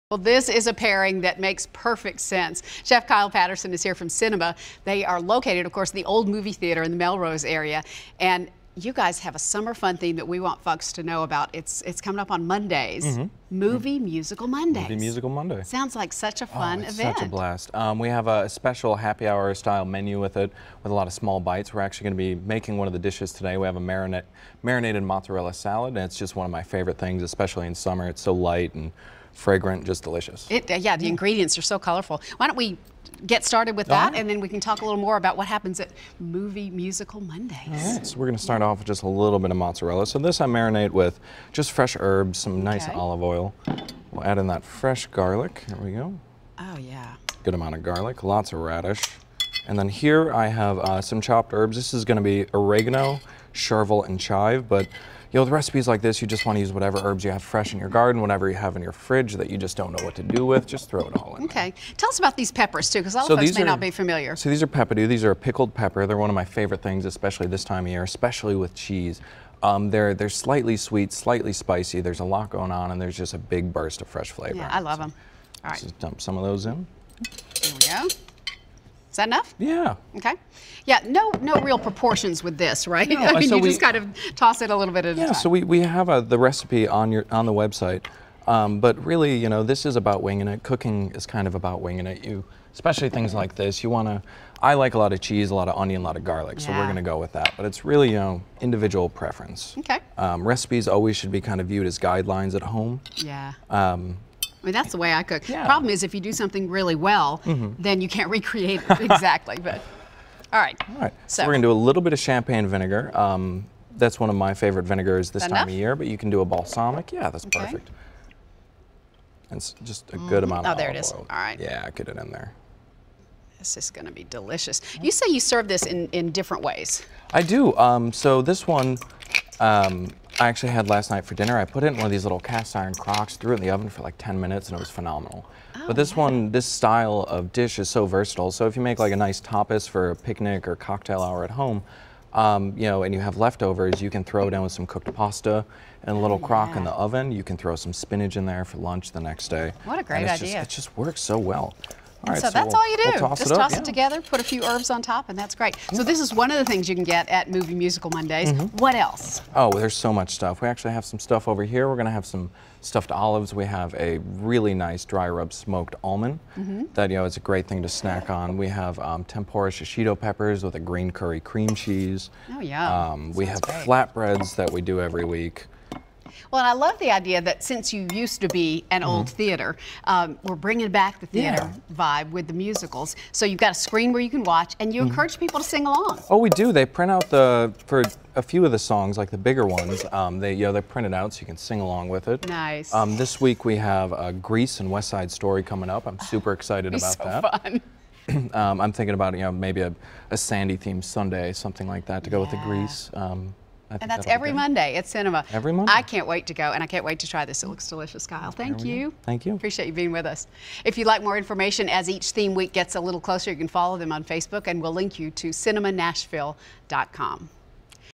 Movie Musical Mondays at Sinema Nashville
Chef Kyle Patterson shares his Marinated Mozzarella  Salad Recipe and Previews Movie Musical Mondays at Sinema Nashville